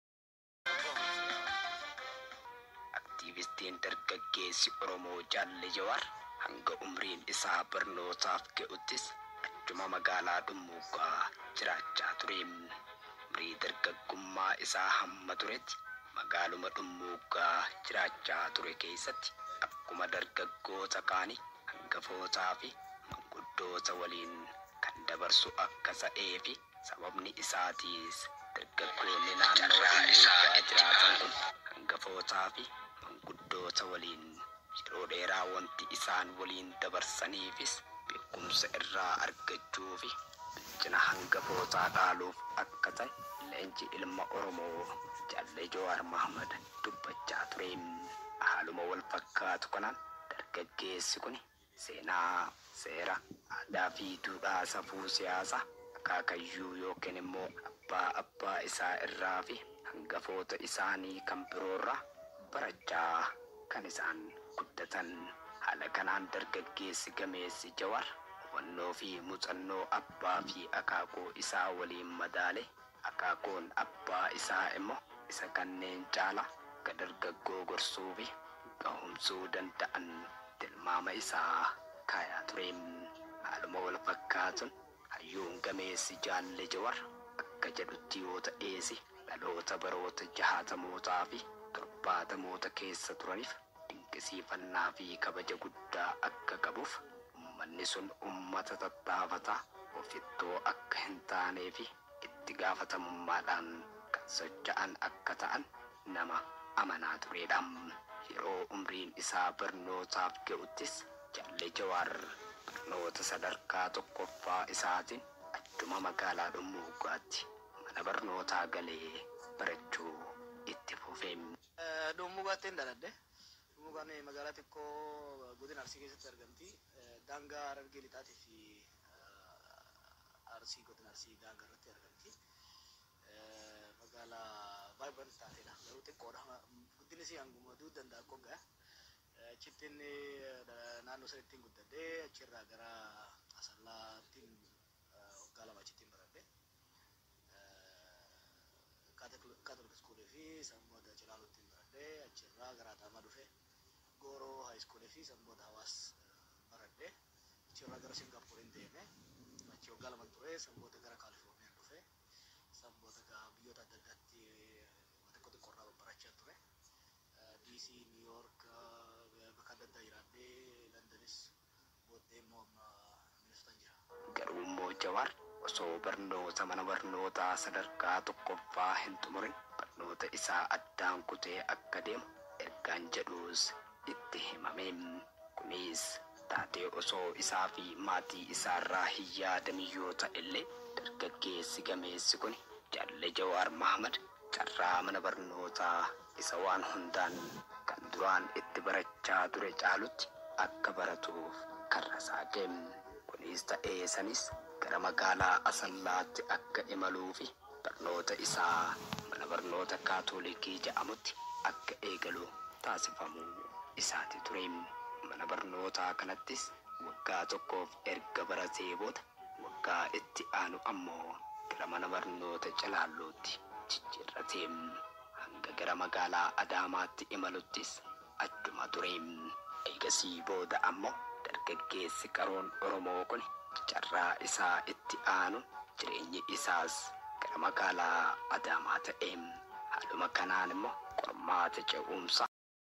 Oromo Nation. History of Jawar Mohammed
Oromo Nation,
Jawar Mohammed is the energetic, dynamic and controversial political face of the majority of young Ethiopian Oromo’s, who now have been given the green light to freely participate in the country’s changing political scene.

It is evident that he now has a wide reaching network that spans across continents, namely, the Americas, Europe, and obviously Africa.

Jawar has perfected the art of disseminating his views to reach his massive audience base through the expert use of the many media outlets, some of which he runs.